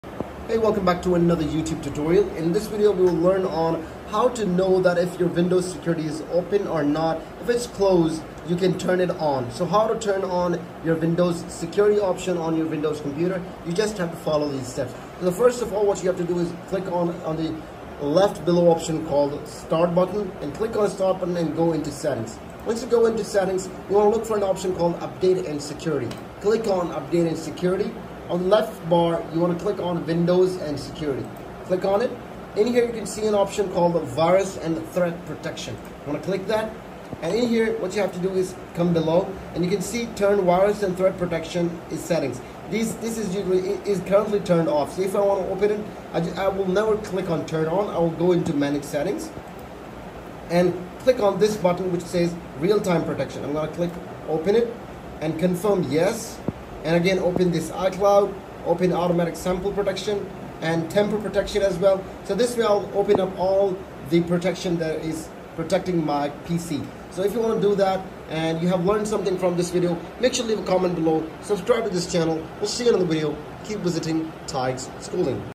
How to turn on windows security on computer
Windows Security is your built-in defense against viruses, malware, and other security threats on your computer. In this tutorial, we'll show you how to turn on Windows Security to keep your PC protected. Whether you're using Windows 10, Windows 8, or Windows 7, enabling Windows Security is essential for safeguarding your data and privacy. Join us as we guide you through the steps to access and activate Windows Security features, including antivirus protection, firewall settings, and more. Don't leave your computer vulnerable—watch this video and strengthen your PC's defenses today!